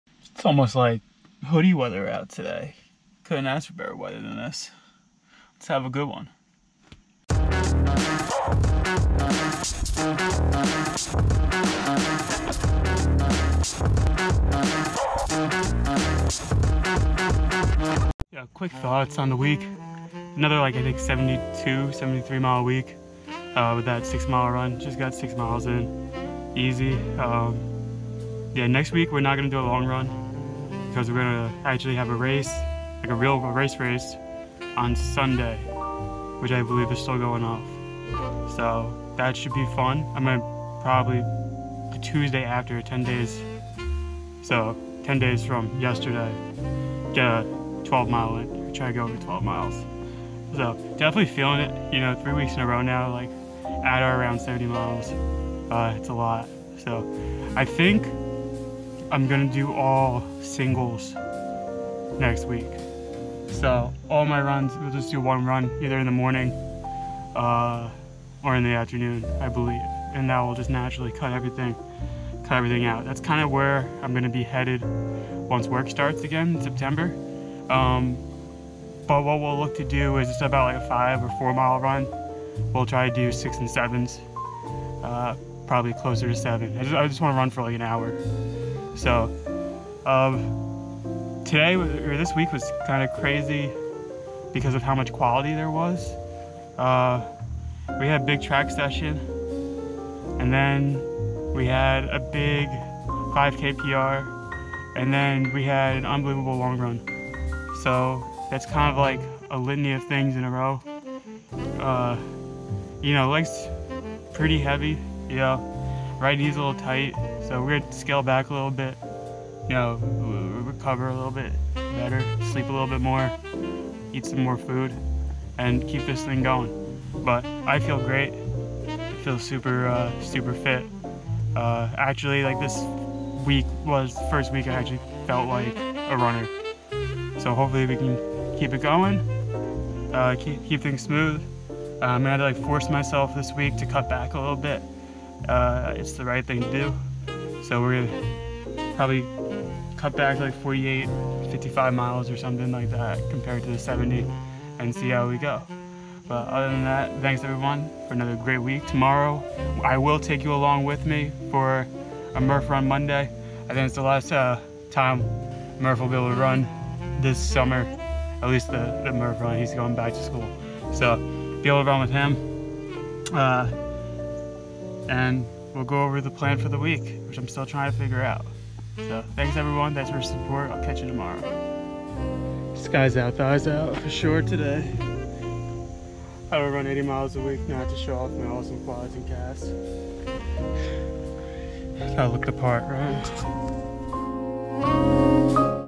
JDProject || Episode 028: Sunday Reflection
Quick video today reflecting on the week.

Really felt like a runner this week.

Going to force myself to take it easy coming up.

Excited for a race in 7 days!

🏃 6.3M Run
👟 New Balance 1080v10
📈 72M on the week

What I'm reading

Can't Hurt Me: Master Your Mind and Defy the Odds - David Goggins